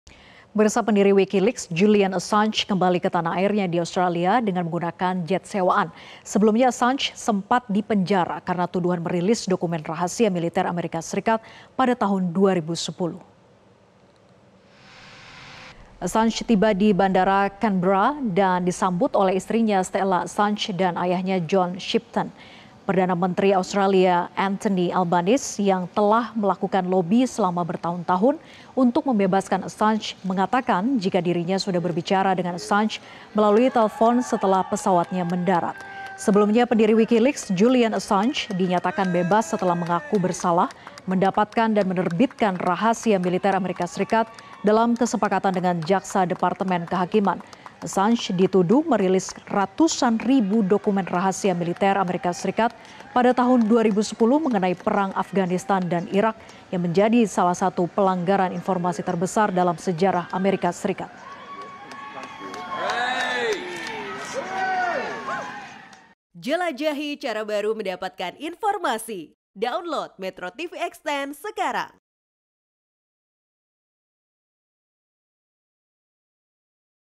Pendiri Wikileaks, Julian Assange Bebas
MetroTV, Pendiri Wikileaks, Jullian Assange kembali ke Australia menggunakan pesawat sewaan. Sebelumnya, dirinyta sempat dipenjara karena tuduhan merilis dokumen rahasia milik Militer Amerika Serikat pada tahun 2010.